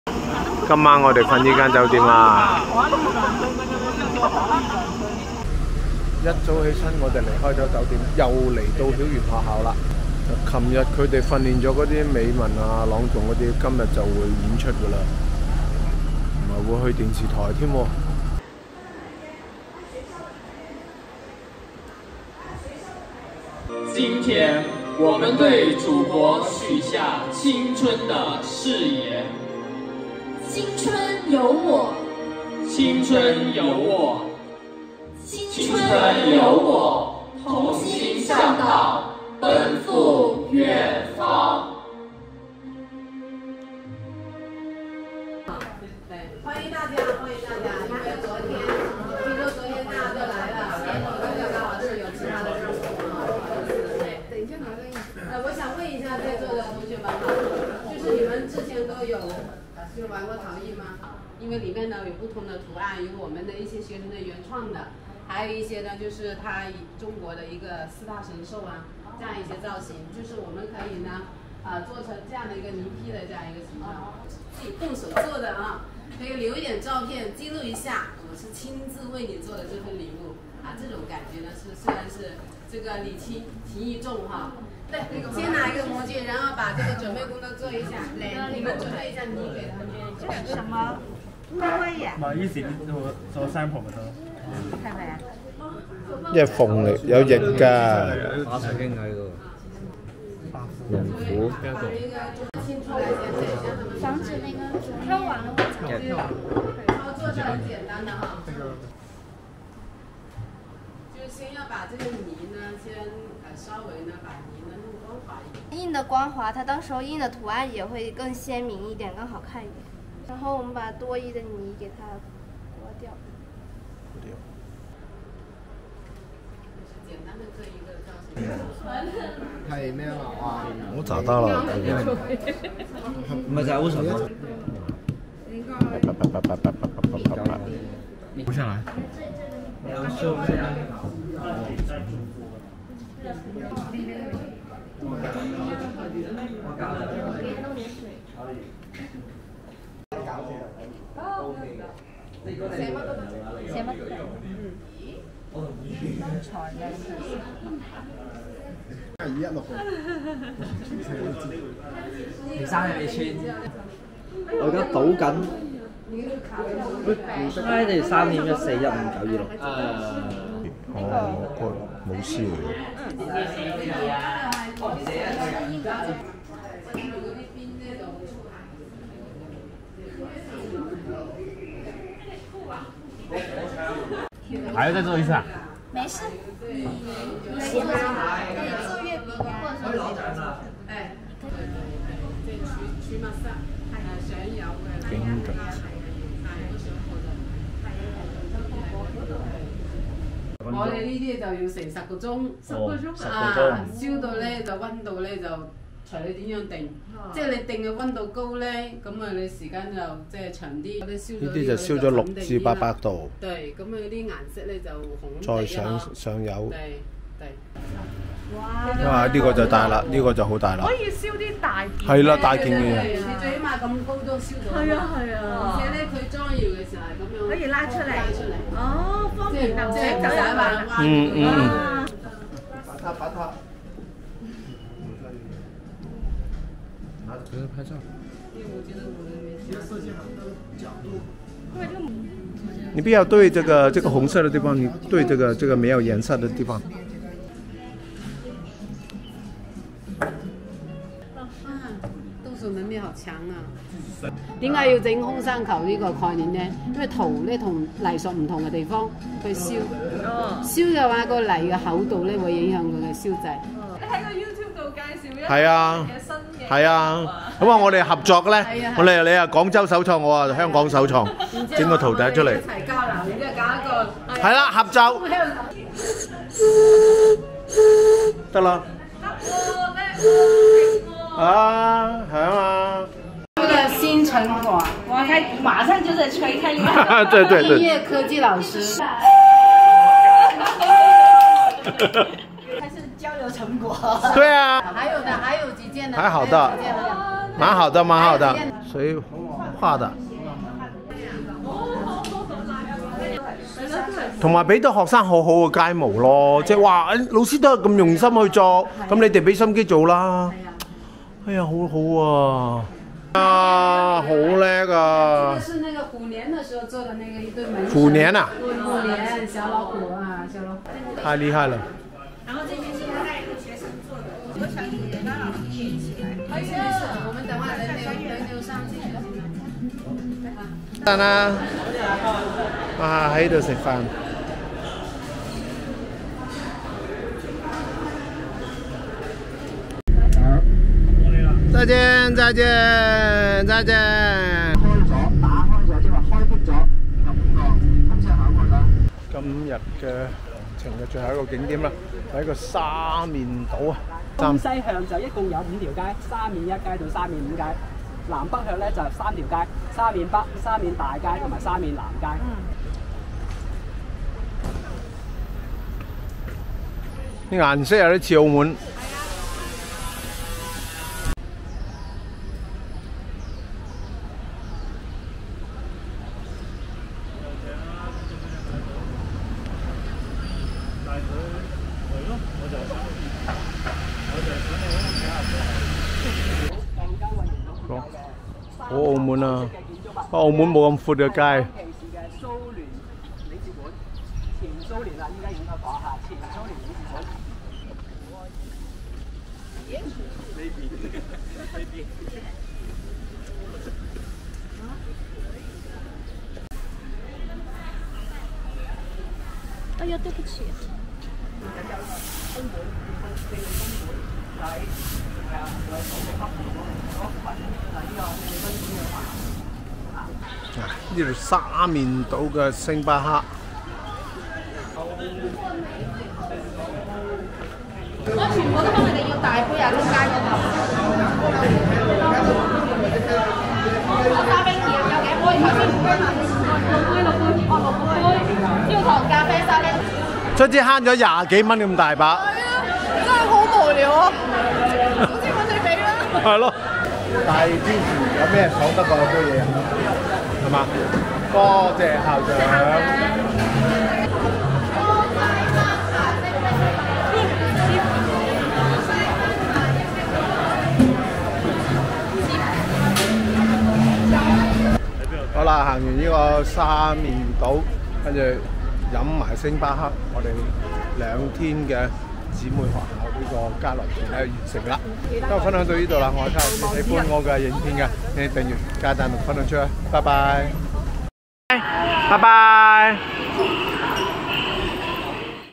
2023_7_5【姊妹廣州市九十七中曉園學校體藝文化交流團】第二天行程
2023_7_5【姊妹廣州市九十七中曉園學校體藝文化交流團】第二天行程

交流團第二天行程更充實，朗誦美文小組往電視台錄影，希望很快看到在電視上播放，其他同學留在曉園學校跟藍老師和曉園同學們學習陶藝。卡洛斯還和藍老師合作製成了陶笛的雛型，相信很快在香港和廣州同時首創。離開曉園到交流團最後一個景點沙面島，島上還有歐陸風的星巴克，校長請全團飲咖啡，謝主隆恩！